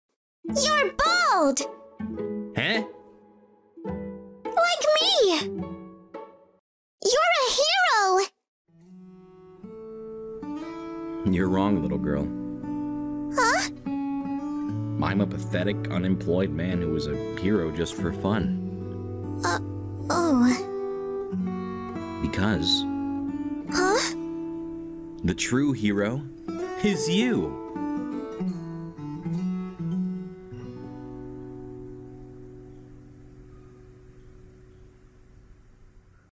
One Punch Man - The true hero is you! feat. ProZD & SHELLAH
An audio video version of a popular One Punch Man fan-art.